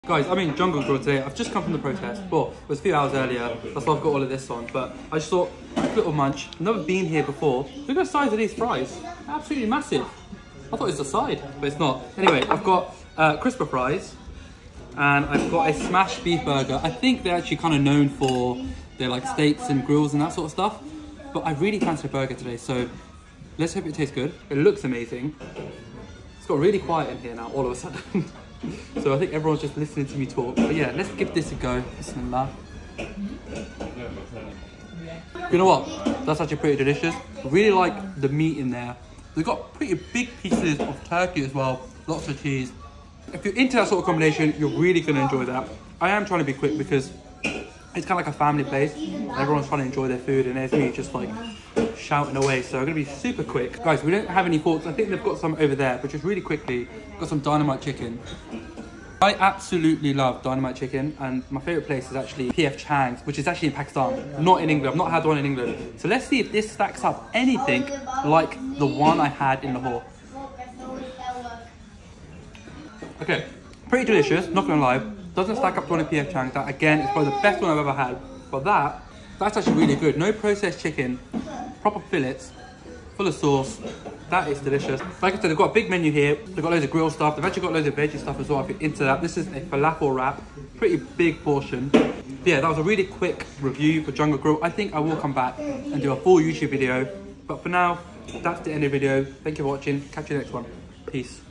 Grill& Burger Spot, Let's Dig In!
We went to Jungle Grill In Manchester to check out their menu.